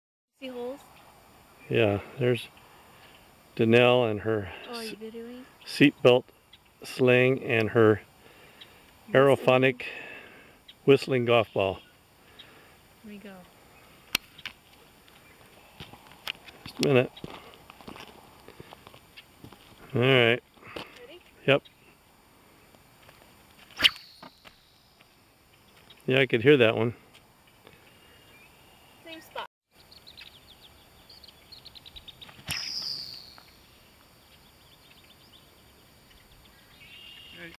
Danelle slinging fig 8.wmv
Danelle slinging fig 8. Throwing a whistling golf ball ala Hondero from slinging.org using a seat belt sling.